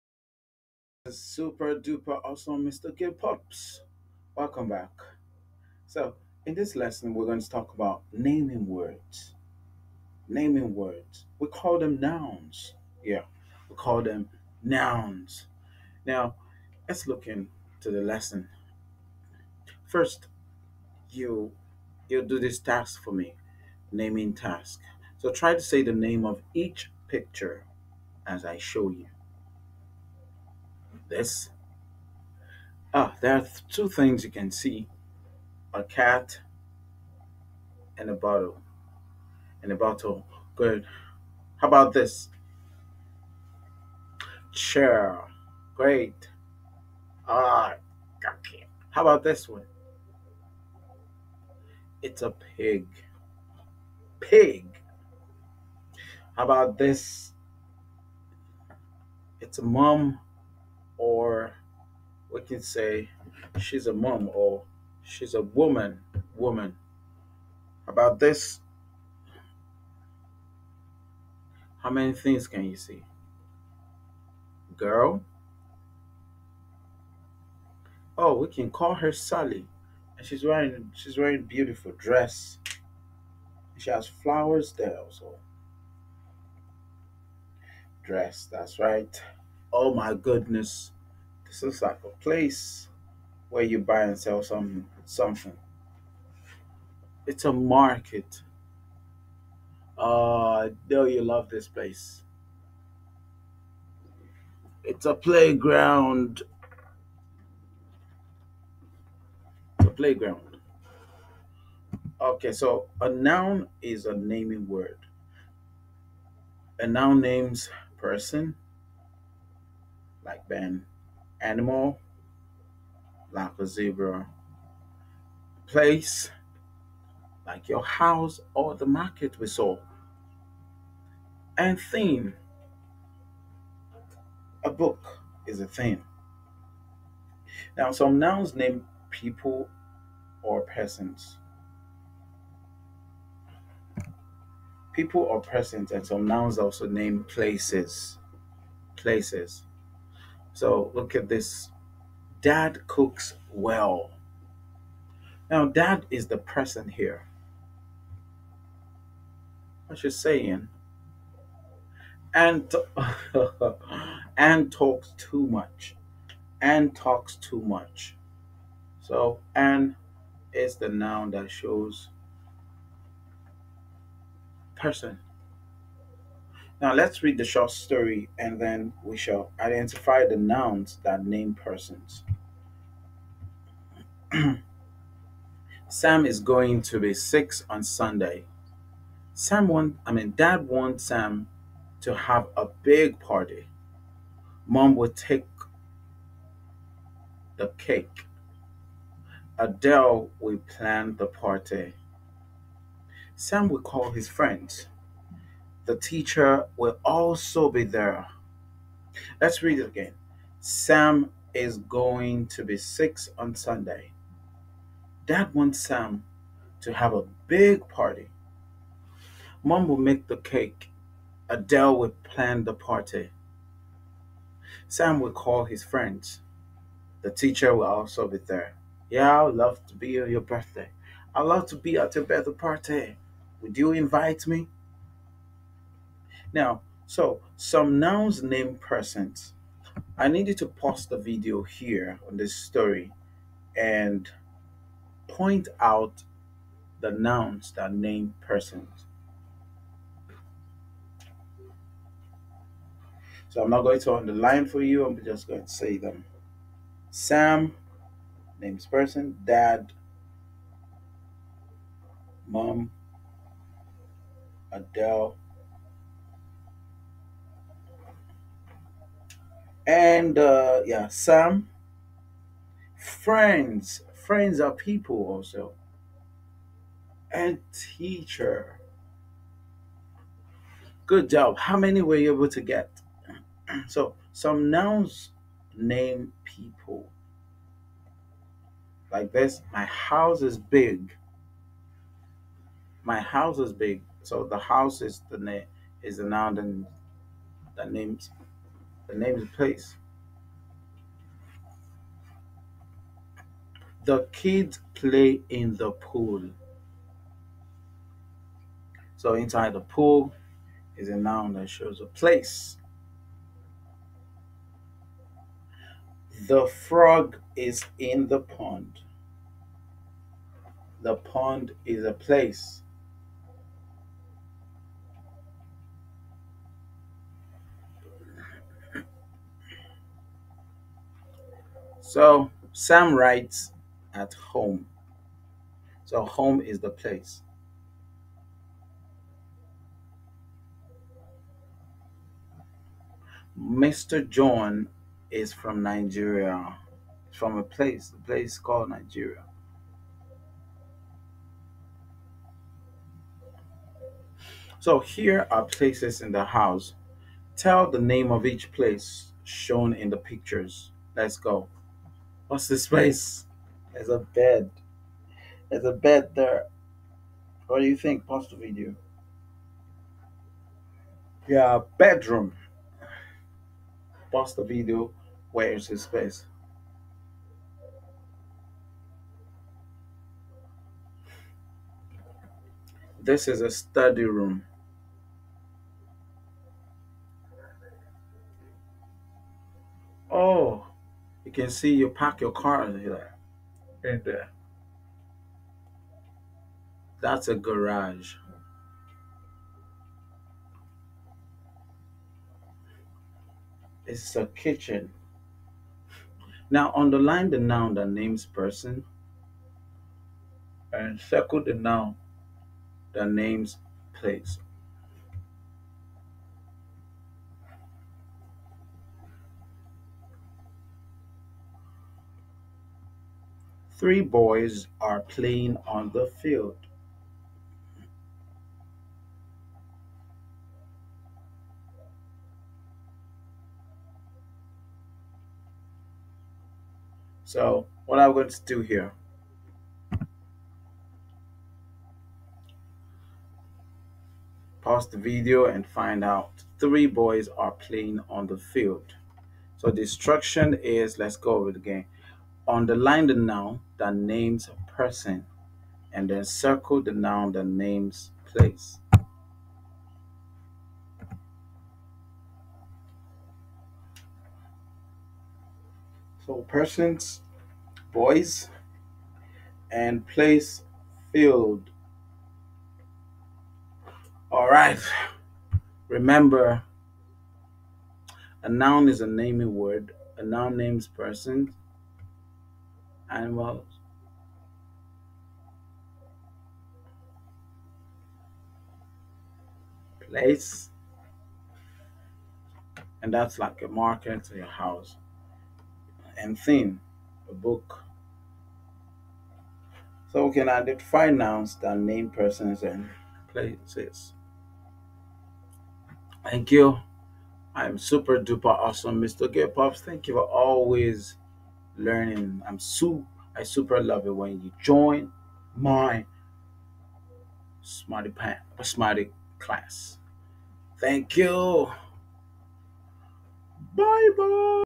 NOUNS PERSONS AND PLACES
In this lesson, we identify nouns that name persons and places.